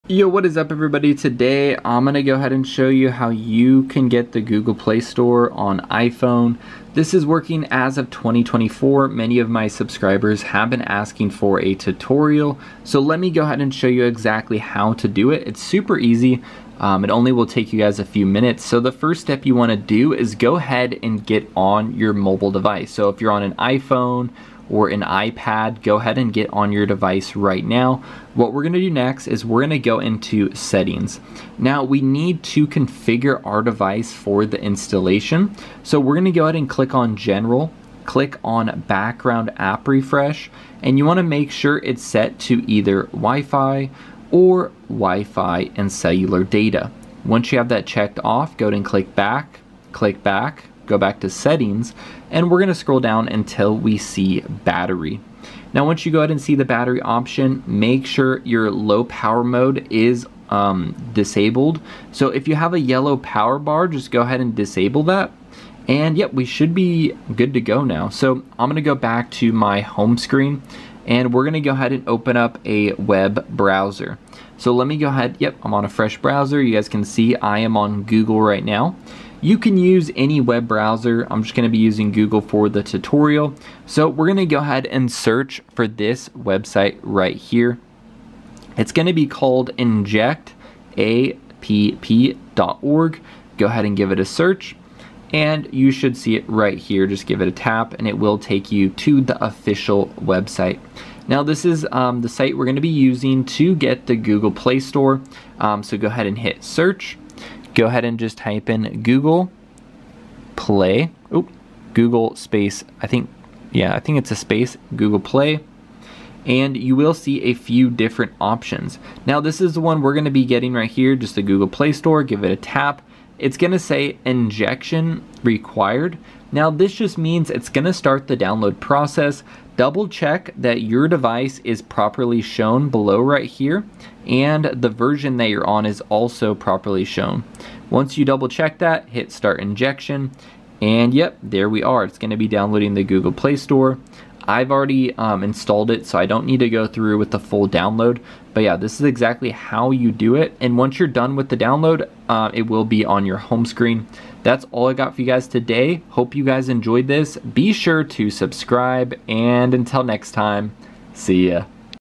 How To Download Google Play Store On iOS iPhone & iPad Google Play Store iPhone Download 2024
Google Play Store iPhone Download 2024 🌟 Discover the secrets to accessing Google Play Store on your iOS devices with ease in 2024! Our guide simplifies the process, ensuring iPhone and iPad users can seamlessly tap into the world of Google Play Store. From crucial tips to detailed, step-by-step instructions, we've got everything you need covered. Envision a realm where your iOS device effortlessly connects you to a vast universe of apps, games, and utilities.